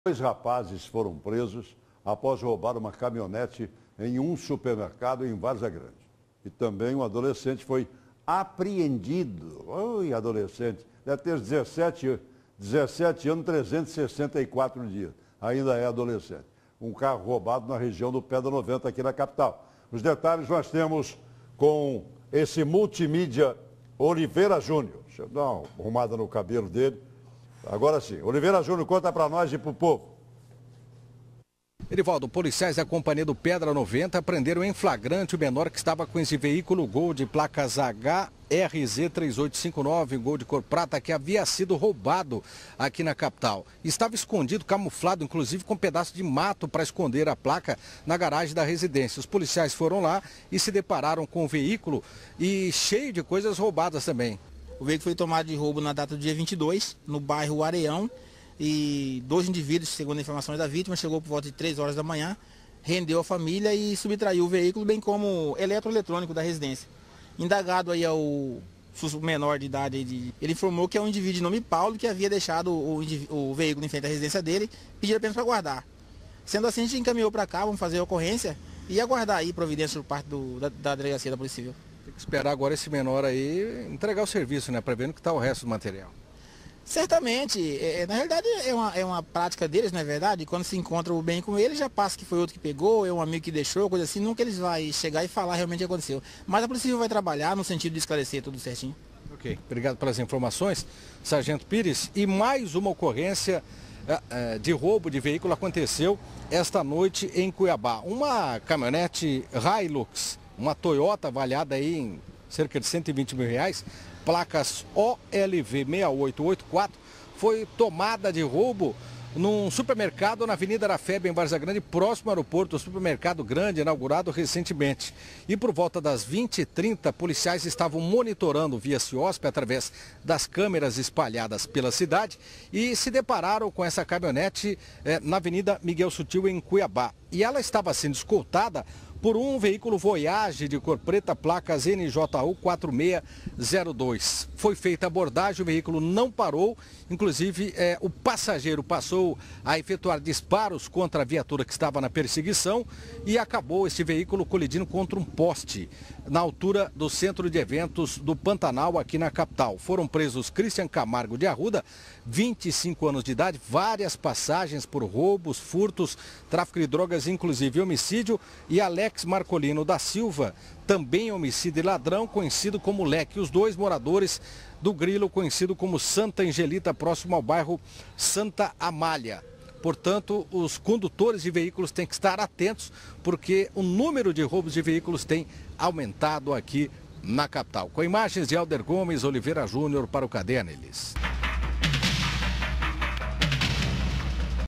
Polícia prende acusados de roubo de veículos em Várzea Grande
Os 2 rapazes é acusado de roubar uma caminhonete em um supermercado.